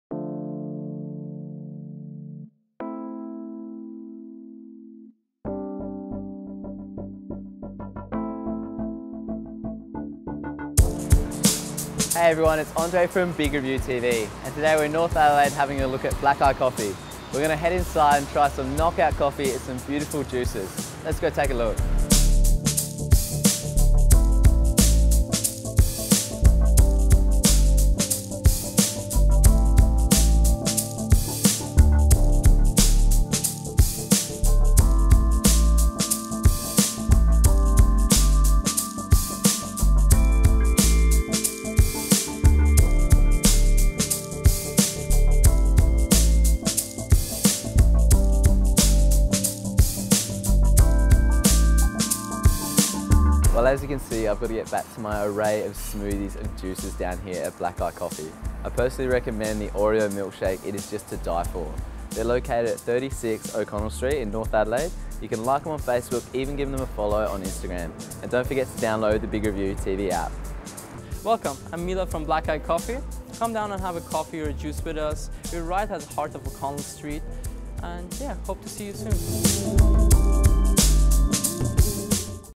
Black Eye Coffee a Cafe in Adelaide serving Coffee, Sandwich and Smoothie
Black Eye Coffee offers fresh juices, smoothies, milkshakes, coffee, tea, sandwiches, and cakes in a nice atmosphere.

Black Eye Coffee
36 O'Connell Street Adelaide SA Australia 5000